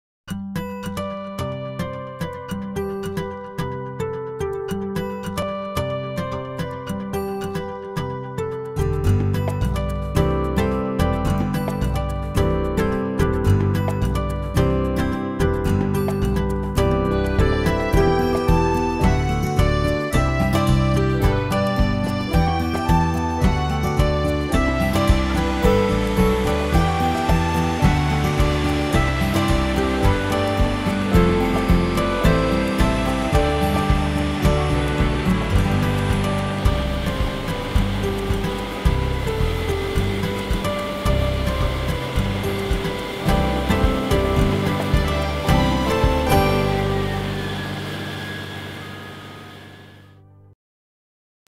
Ohio Wheat Harvest short (shot and edited by Tom Hofbauer)
This is a very short film celebrating the tradition of the wheat harvest in NW Ohio.